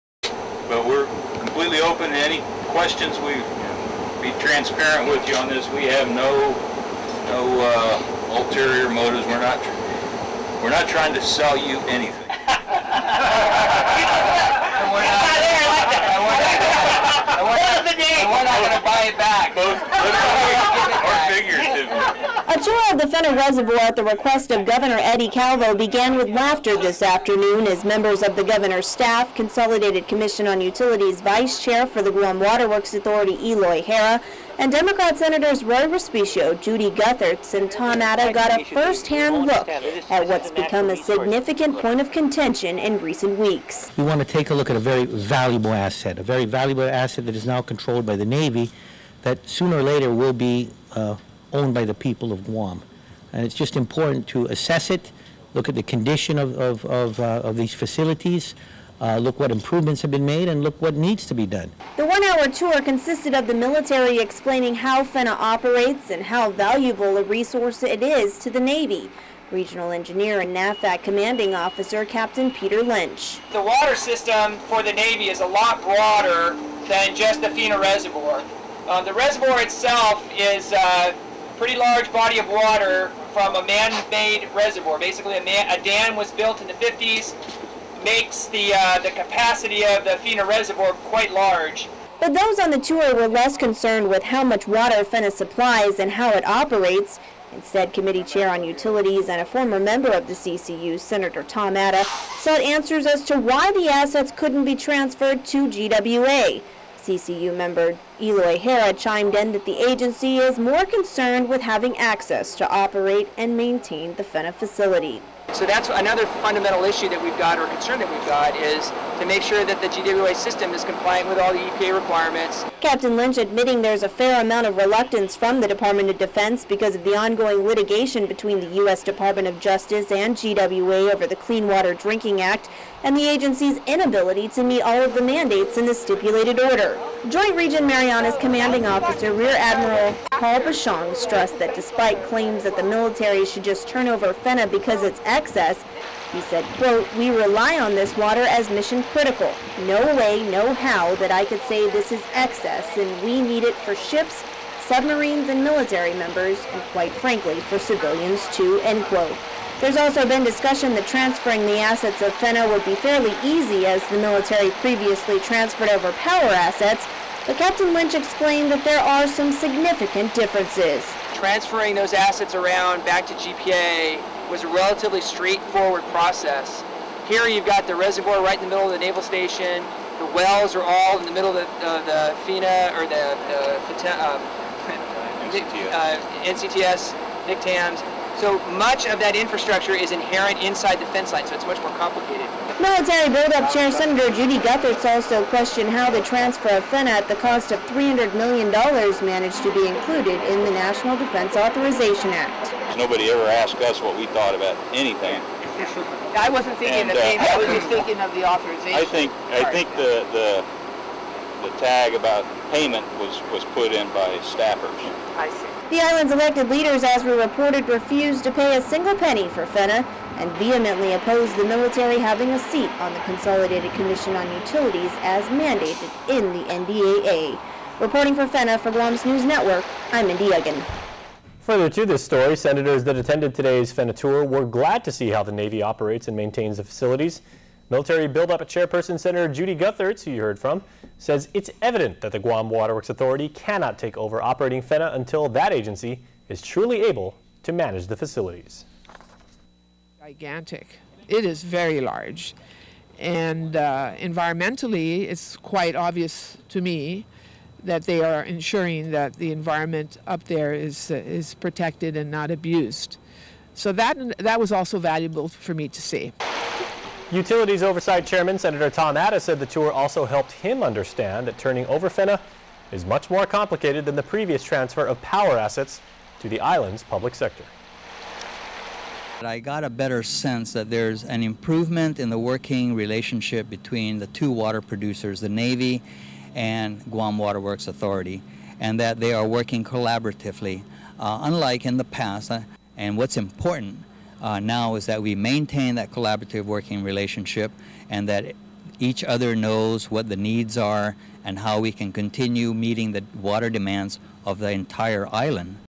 Military officials give tour of Fena to GovGuam officials
Just a week after members of the 31st Guam Legislature joined with Congresswoman Madeliene Bordallo to march to the gates of Fena, Governor Eddie Calvo and several senators joined the military for a tour of the Navy's water and waste water system inside the gates.